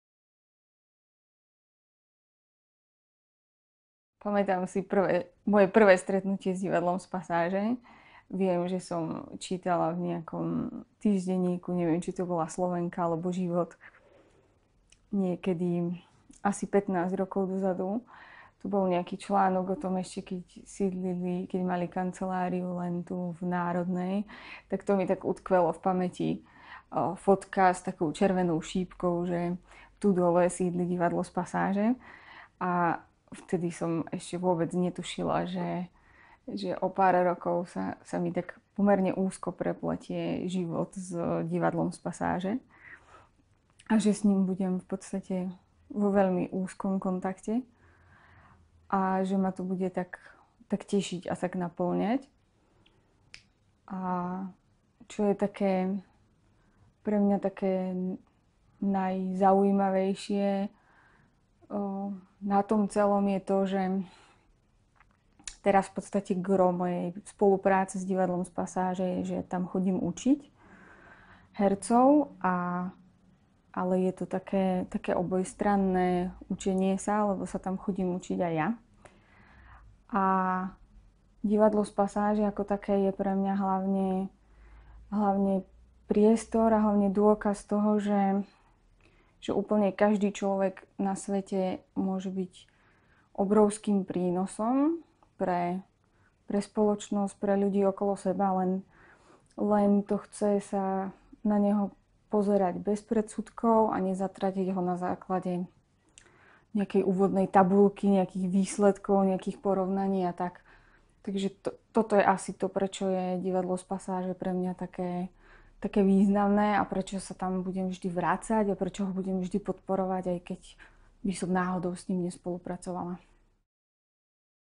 Zuzana Úradníčková
20 rokov Divadla z Pasáže
20... a predsa sa hrá

ROZHOVORY O PASÁŽI

KAMERA: Peter Vlkovíč, Monika Vaškorová
RÉŽIA A POSTPRODUKCIA: Michal Kočiš